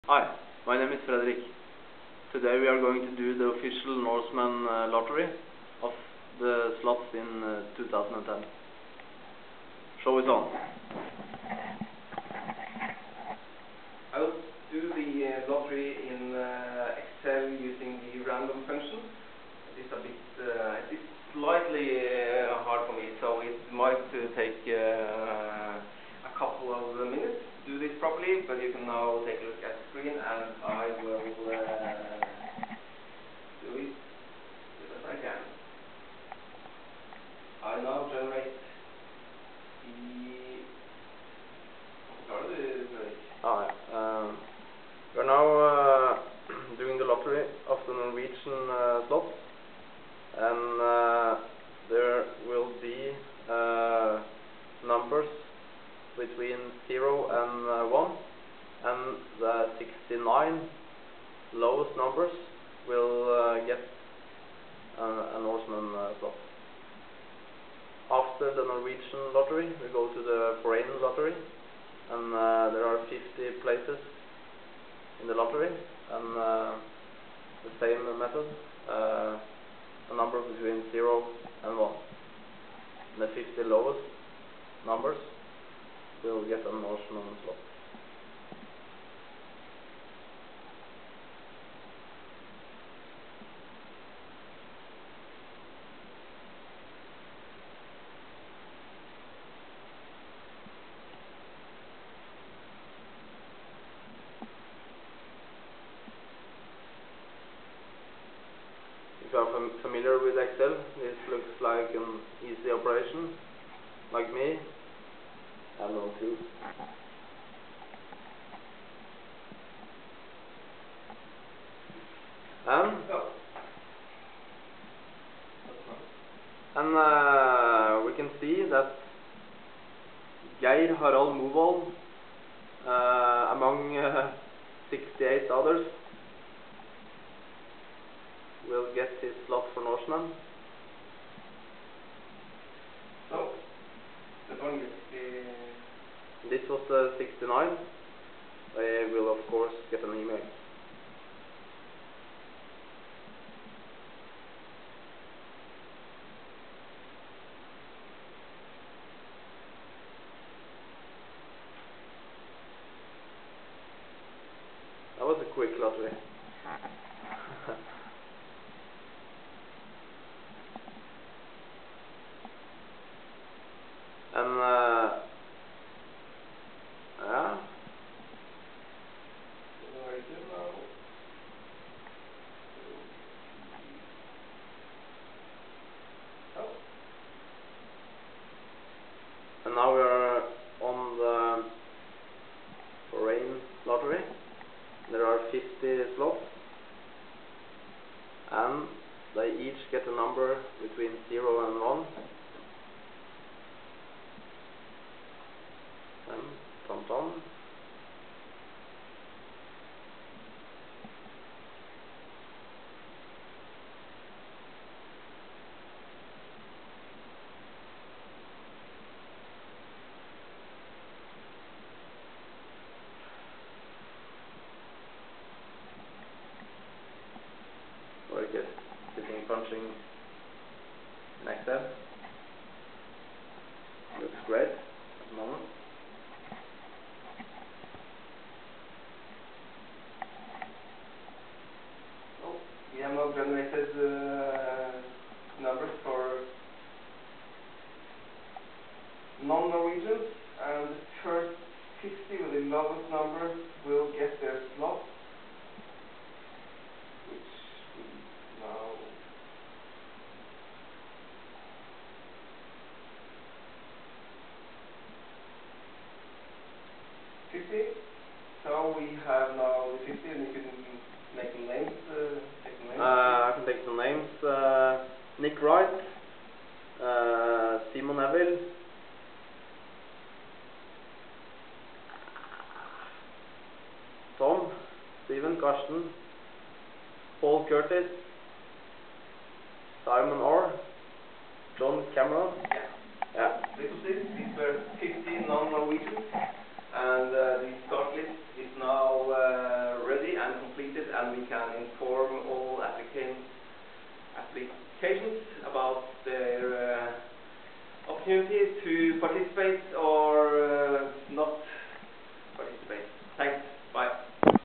Norseman 2010 Lottery
This video is published to document the lottery for the Norseman Xtreme Triathlon 2010.

It is a very low quality video and only of interest for the most interested followers of the event. Sorry about showing my belly!